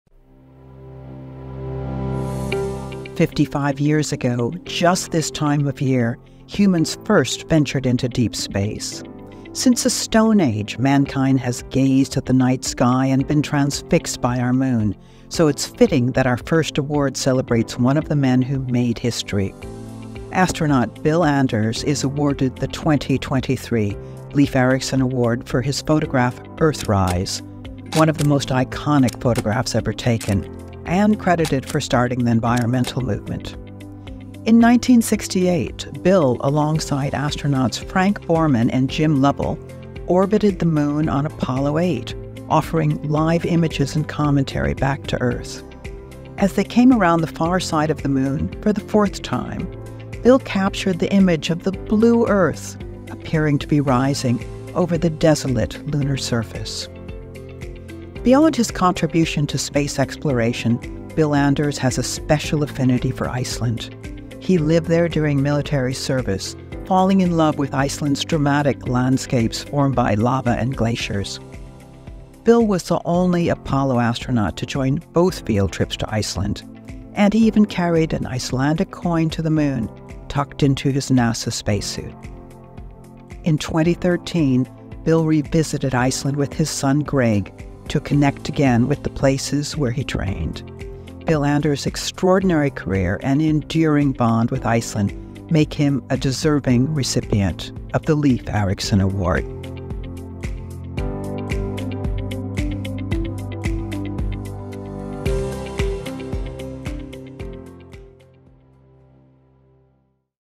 Apollo 8 Astronaut Bill Anders - 2023 Leif Erikson Award
Astronaut Bill Anders is awarded the 2023 Leif Erikson Award for his photograph Earthrise, one of the most influential environmental photographs ever taken.

In 1968, Bill became the first person to fly to the Moon alongside astronauts Frank Borman and Jim Lovell on NASA's Apollo 8 mission. The three astronauts circled the Moon ten times, offering live images and commentary back to Earth. Coming around from the far side of the Moon for the fourth time, Bill captured the iconic picture of the Earth, appearing to be rising over the Lunar surface.

Beyond his contributions to space exploration, Bill Anders has a special affinity for Iceland. He lived there during military service, appreciating its captivating landscapes. He was the only Apollo astronaut to join two field trips to Iceland, and he even carried an Icelandic 25 aurar coin to the Moon in his Apollo 8 mission. In 2013, he revisited Iceland with his son Greg to reconnect with the places he trained. Bill Anders' extraordinary career and enduring bond with Iceland make him a deserving recipient of the Leif Erikson Award.